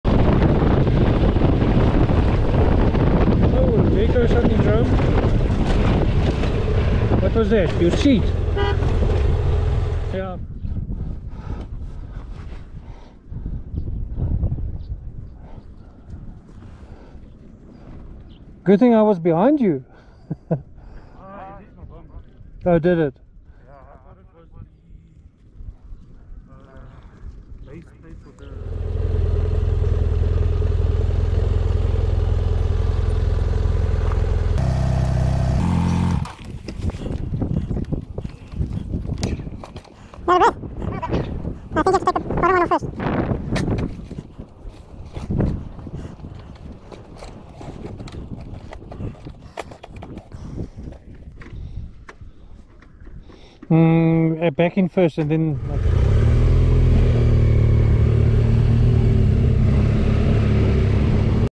R1200GS Lost its seat
While traveling back to Thaba-Tseka from Katse dam in December, the R1200GS lost its seat on a bump.